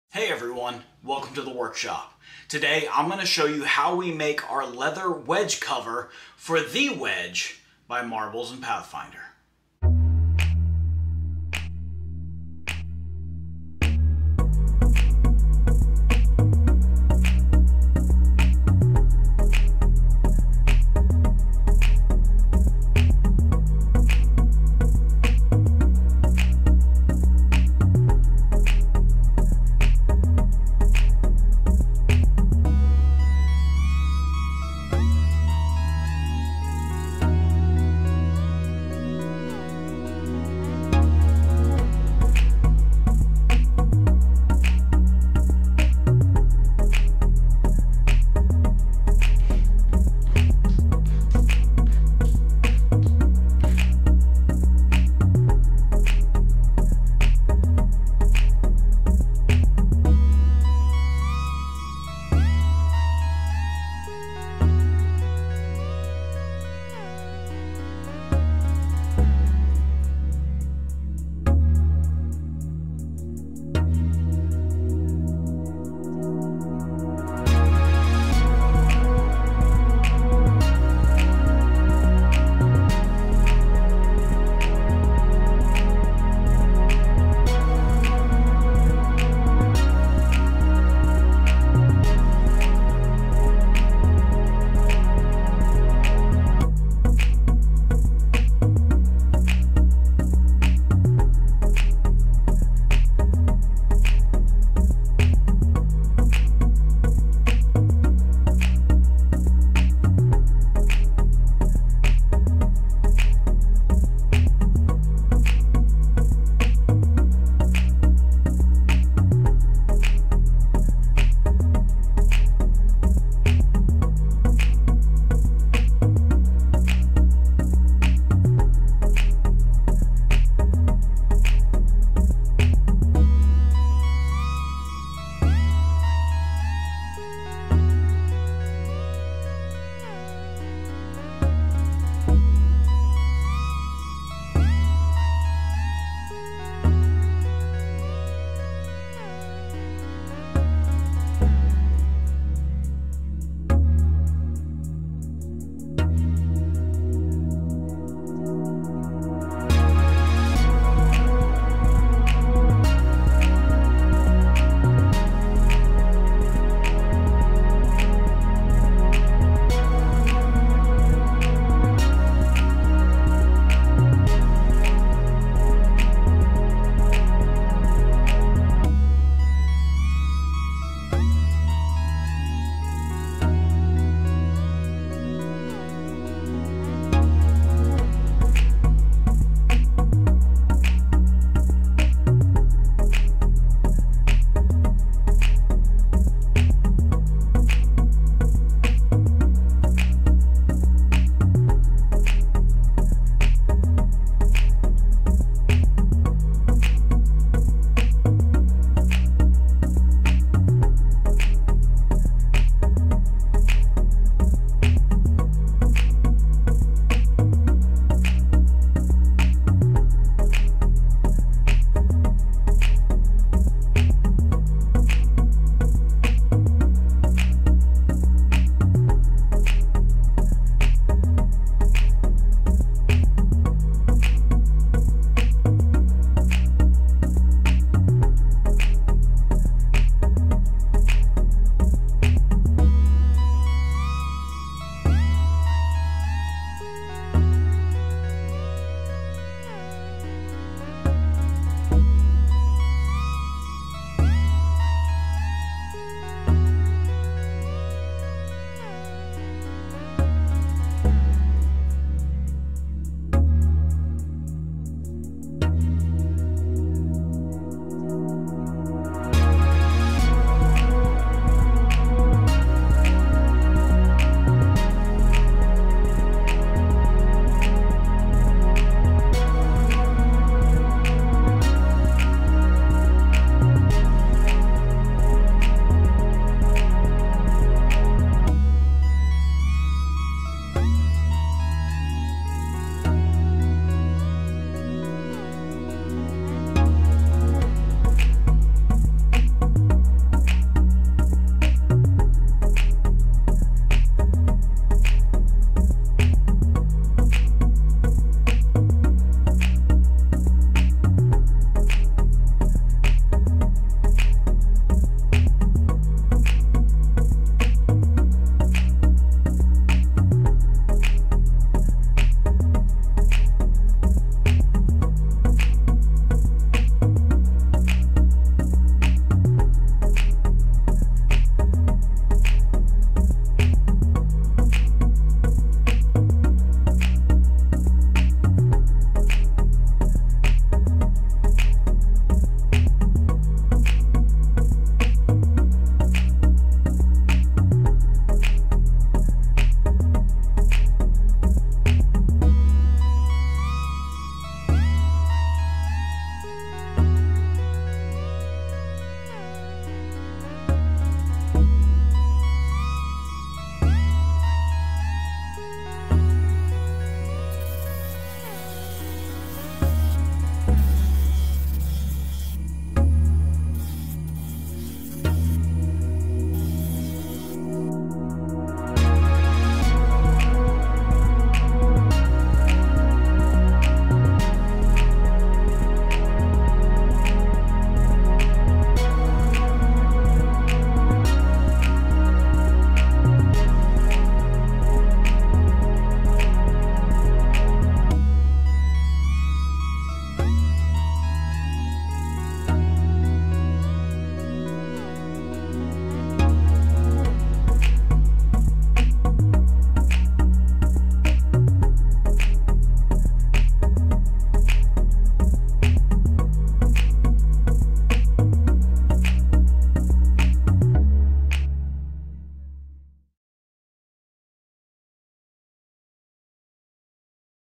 Leather Wedge Cover Build for the Wedge by Marbles and Pathfinder
See how we make our Leather Wedge Cover made to fit the Wedge by Marbles and Pathfinder.